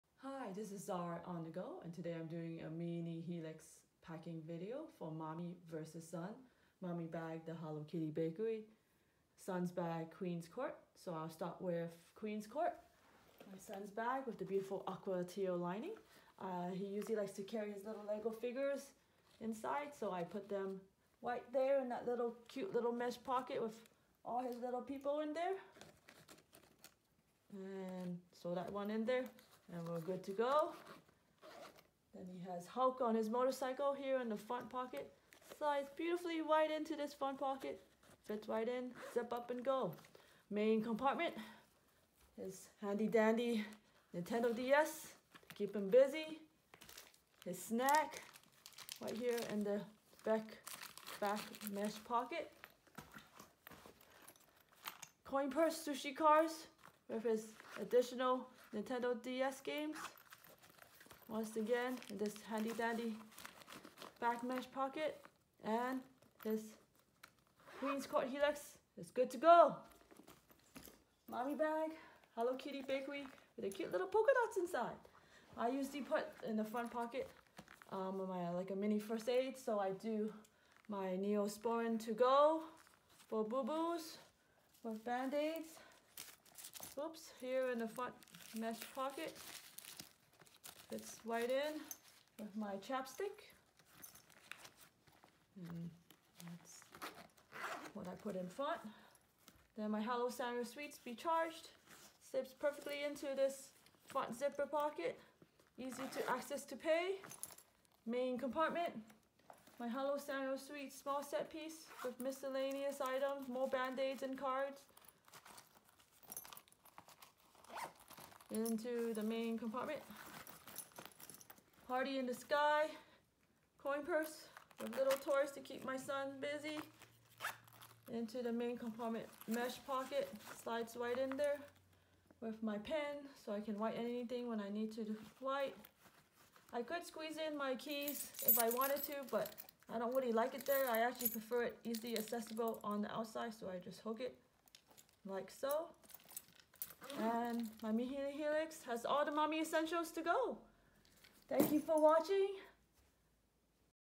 Updated Jujube Mini Helix Mom v.s. Son
Hi! I'll love to share our love for the mini helix in our family - how mommy packs versus how my 9 years old son packs. Such a versatile bag! We love it out and about!